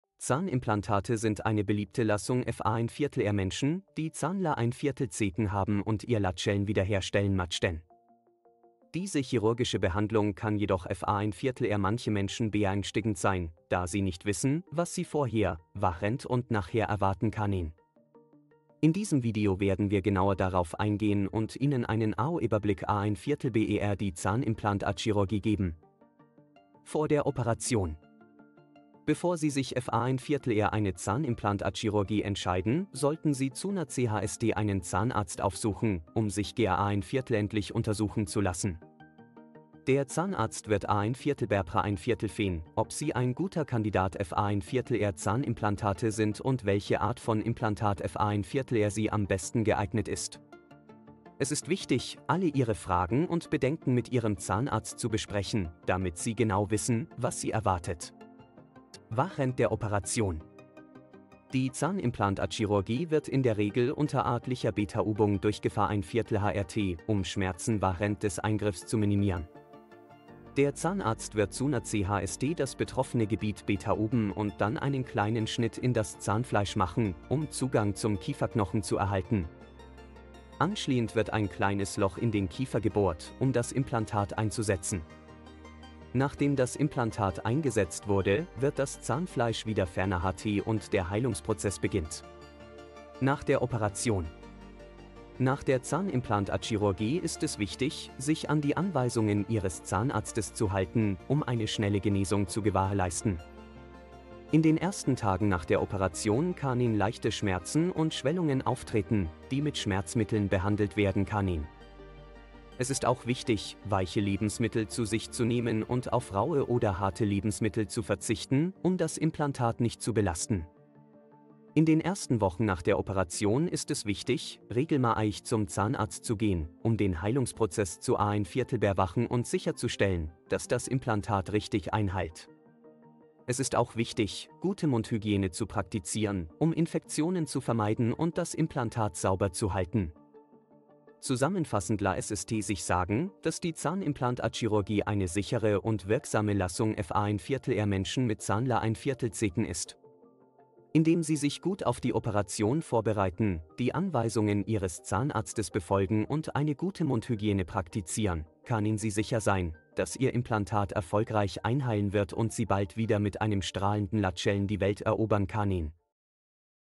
Zahnimplantate - Wie läuft die Behandlung ab?
Zahnimplantate sind eine beliebte Lösung für Menschen, die Zahnlücken haben und ihr Lächeln wiederherstellen möchten. Diese chirurgische Behandlung kann jedoch für manche Menschen beängstigend sein, da sie nicht wissen, was sie vorher, während und nachher erwarten können. In diesem Video werden wir genauer darauf eingehen und Ihnen einen Überblick über die Zahnimplantatchirurgie geben. Zahnimplantate, Zahnlücken, Lächeln, chirurgische Behandlung, beängstigend, vorher, während, na